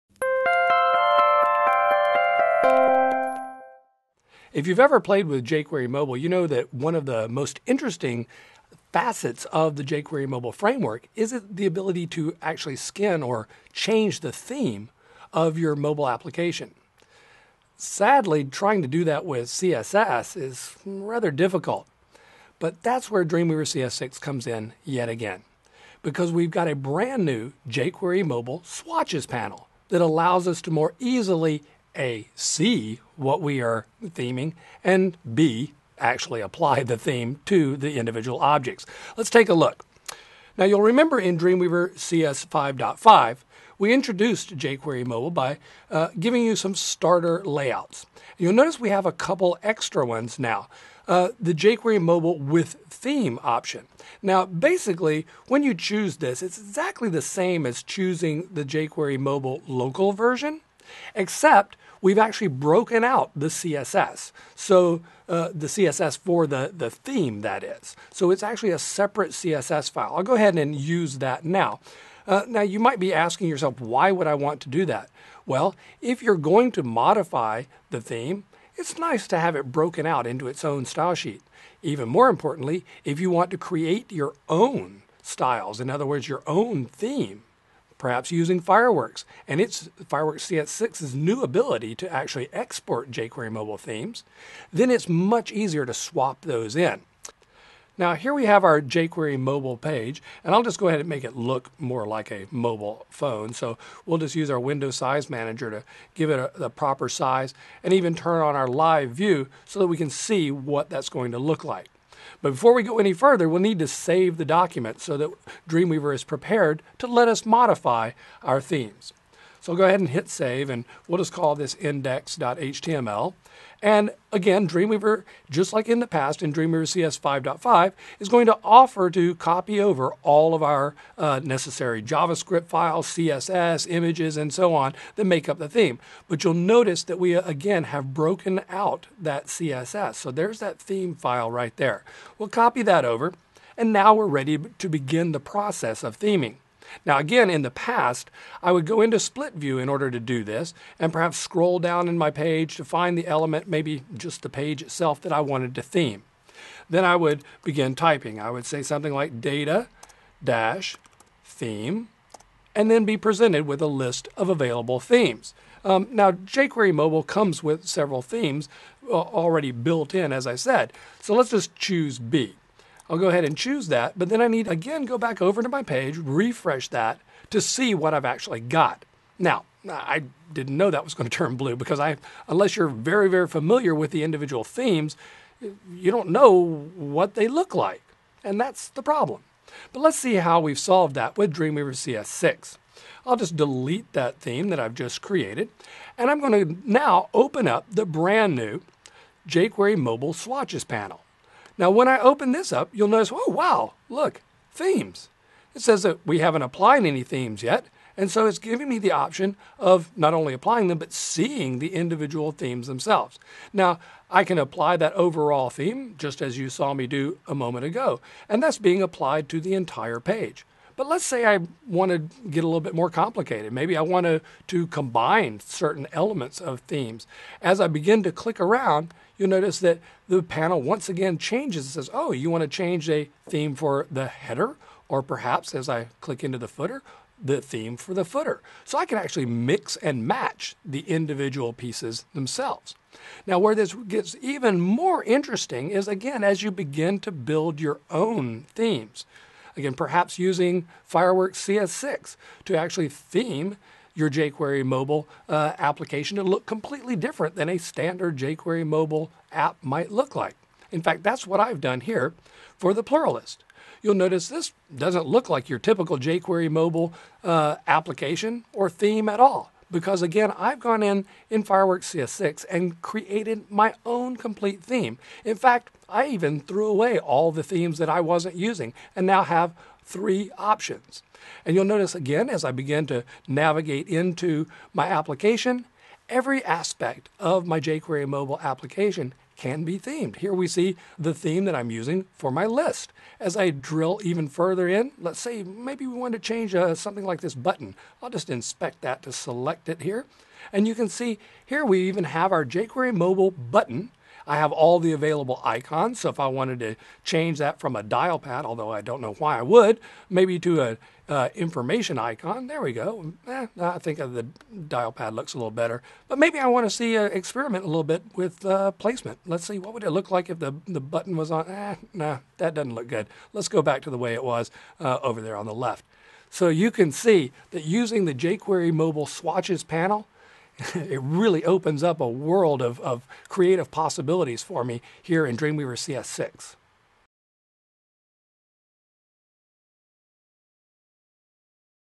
Using JQuery Mobile Swatches in Dreamweaver CS6 to Easily Skin Mobile Applications
Adobe Evangelist Greg Rewis shows how to easily skin mobile applications using Dreamweaver CS6.